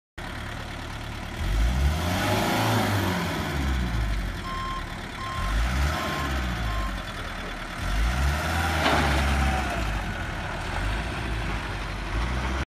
1995 GMC W4 For Sale
16' Box w/Roll Up Door, 14250# GVWR, 235 CID Isuzu  48D2TC Gas, Automatic, S/N: J8DC4B1K3S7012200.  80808 Miles Indicated.

 [GMC W4 BOX VAN TRUCK,]